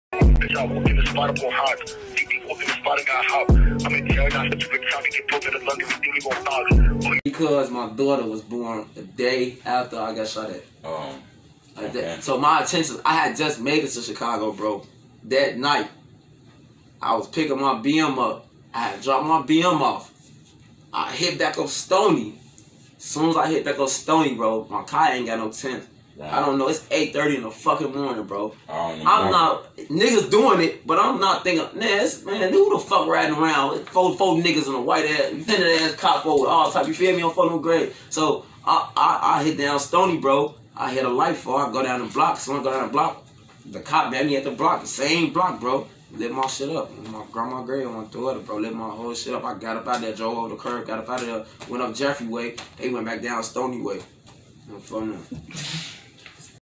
(Fbg) fyndee boy testifies he was shot because he did not have (tints)
interview

witness stand